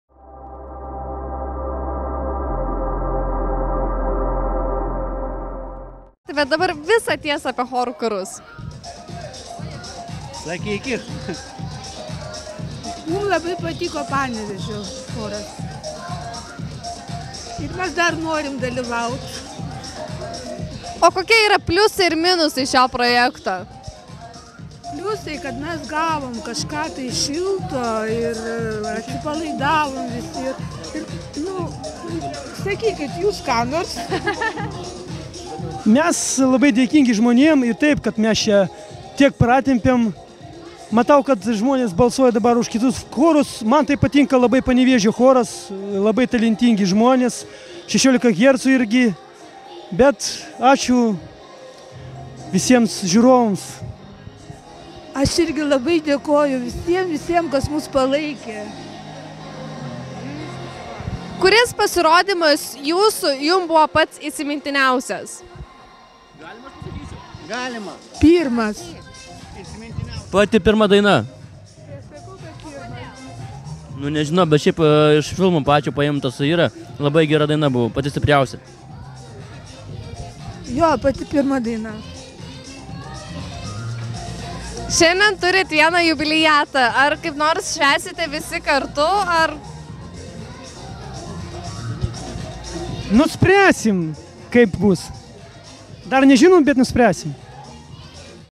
Benamių choras: mes labai dėkingi žmonėms, kad tiek „pratempėme" (interviu)
Dainininkas Stano į muzikinės veiklos sandėliuką gali pasidėti dar vieną skrynutę: subūręs neeilinį, benamių, chorą vyras pasiekė išties nemažais. Prieš metus Stano pasirodymams ruošė moterų kalėjimo gyventojas, lyg to būtų buvę mažai, šiais metais ėmėsi didesnio iššūkio -- benamių. Bet, kaip pats sakė, jokie jie ne benamiai - jie tokie patys, kaip ir mes, tik nublokšti patirtų sunkumų.

Dalyvaudami projekte „Chorų karai" Spindulio choro nariai iškrito tik prieš pat finalą. Visą laiką gavę didžiausią žiūrovų palaikymą, sekmadienio vakarą jo nesulaukė: tiek žiūrovai, tiek komisija jiems atidavė paskutinę vietą.

Po pasirodymo, atliekant egzamino dainą, tiesioginiame eteryje Stano juokavo: „Chebra", sudainuokite taip, lyg tai darytumėte paskutinį vakarą, nes, greičiausiai, toks jums šiandien jis ir bus."

Skaitykite daugiau: Benamių choras: mes labai dėkingi žmonėms, kad tiek „pratempėme" - Naujienų portalas | Alfa.lt